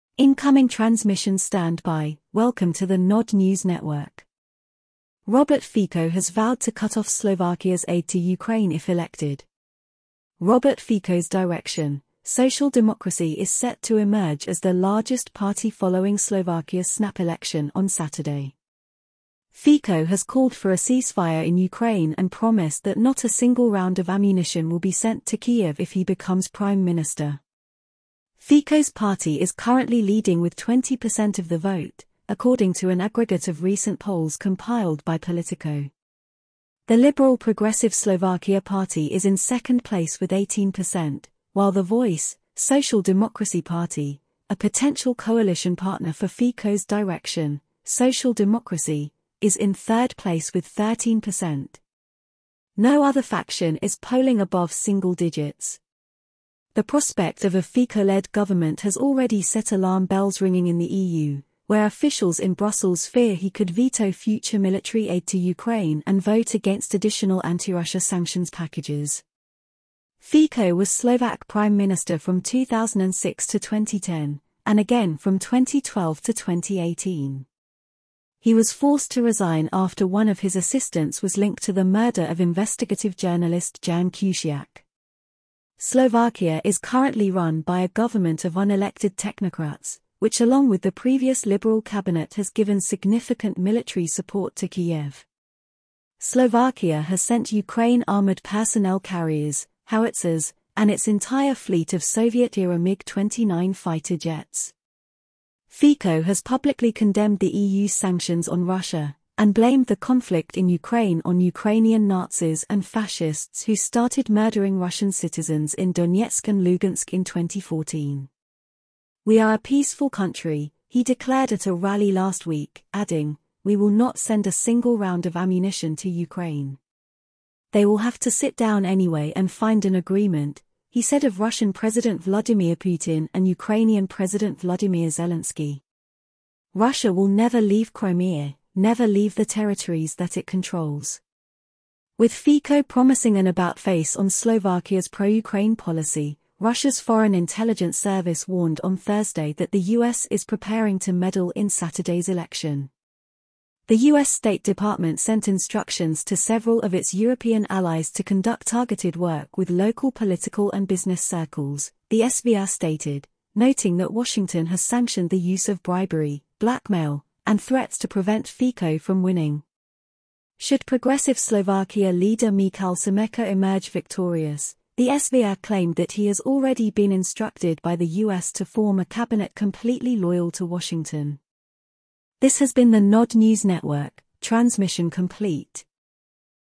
NATO country’s pro Ukraine government to lose election – polls
Robert Fico’s Direction – Social Democracy is set to emerge as the largest party following Slovakia’s snap election on Saturday. Fico has called for a ceasefire in Ukraine and promised that not “a single round” of ammunition will be sent to Kiev if he becomes prime minister.